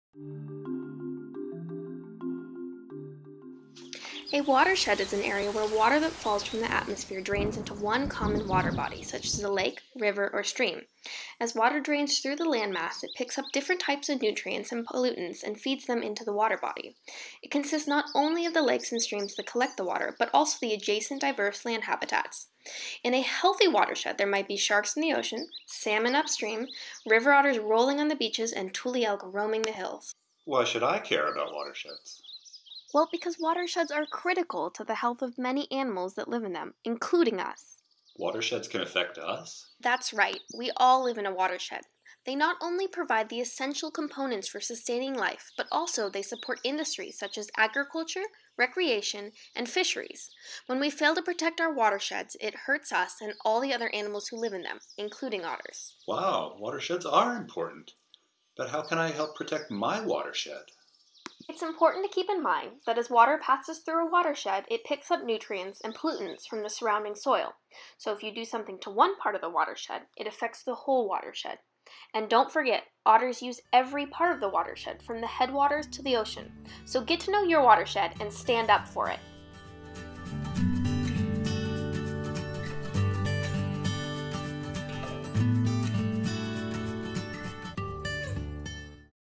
Why are Watersheds important?
Take a brief tour of a watershed and find out why it matters that our shared watersheds are healthy and vibrant!